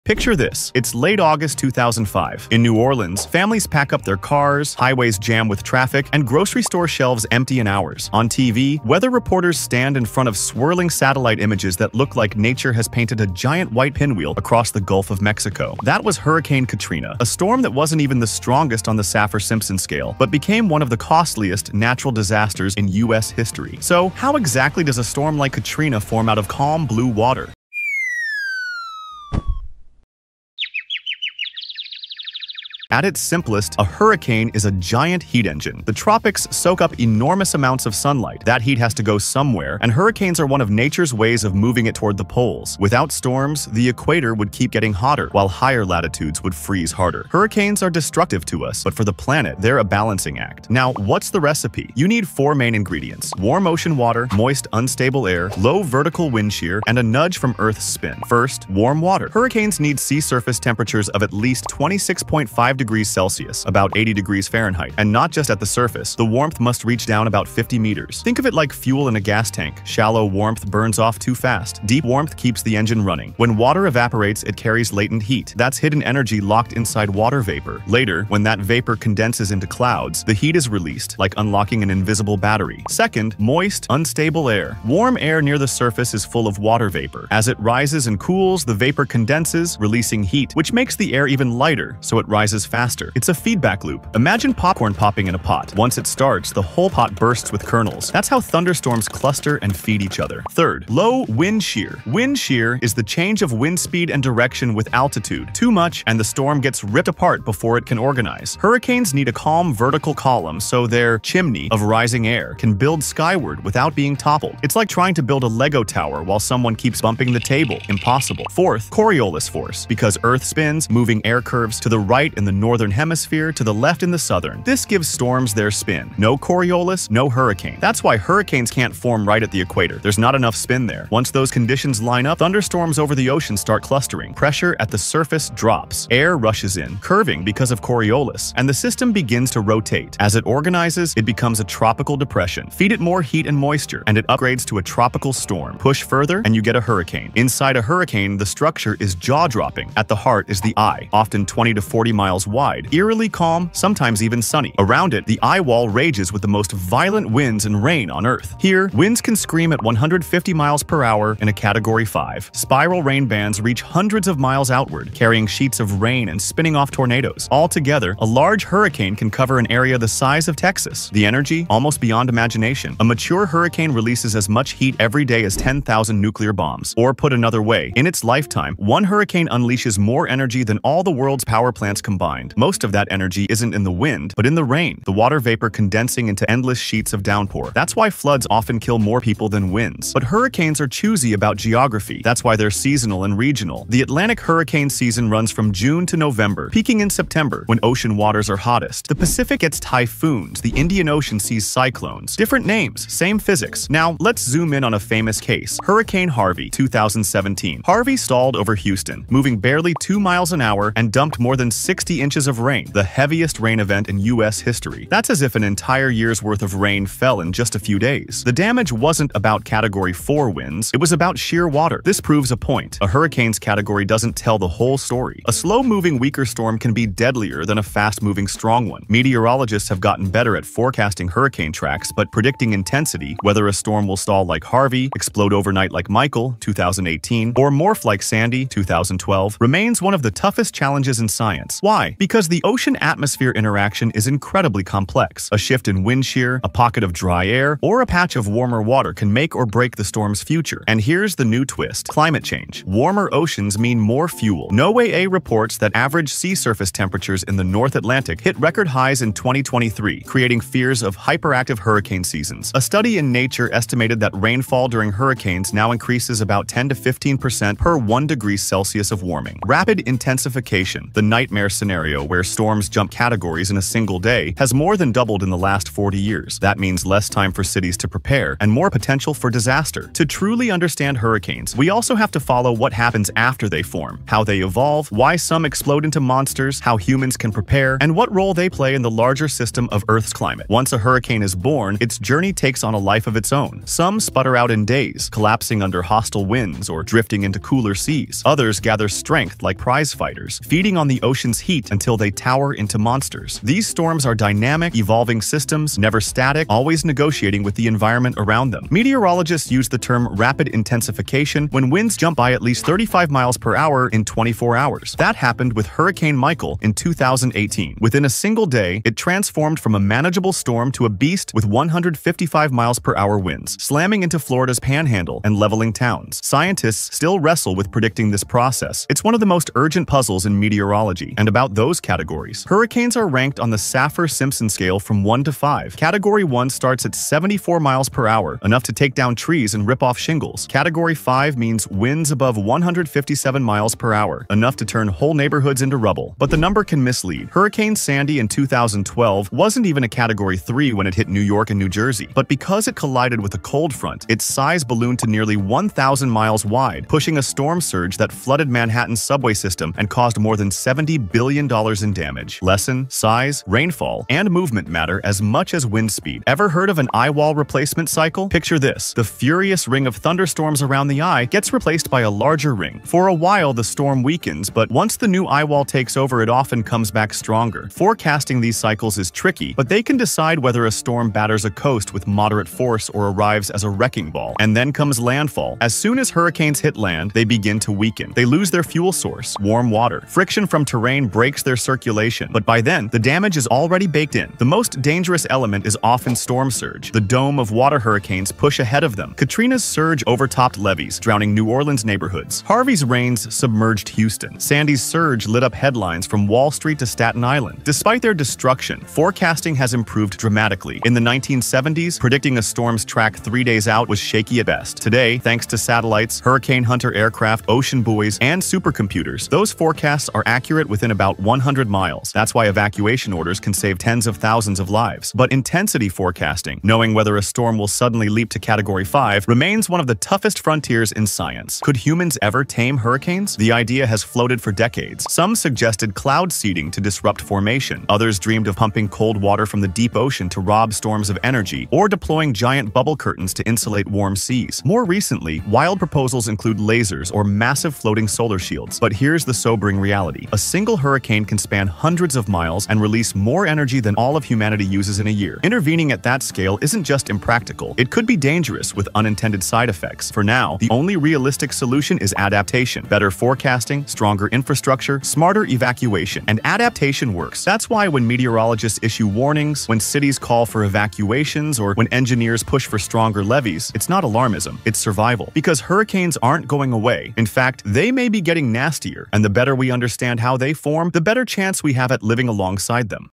How Are Hurricanes Formed?
Every year, massive spirals of wind and water tear across oceans, bringing destruction to everything in their path. But behind the chaos, hurricanes follow precise laws of nature. So how does a simple cluster of thunderstorms grow into one of the most powerful forces on Earth?

In this video, we uncover the science behind how hurricanes are born, grow, and move.

You’ll discover:

Why warm ocean water is the fuel that powers every hurricane
How Earth’s rotation turns storms into spinning giants
Why some storms fizzle out while others become catastrophic
The role of wind patterns and pressure systems in steering hurricanes
What scientists are learning about climate change and the future of storm intensity

Watch now to see how heat, air, and the laws of physics combine to create nature’s most breathtaking and destructive phenomenon.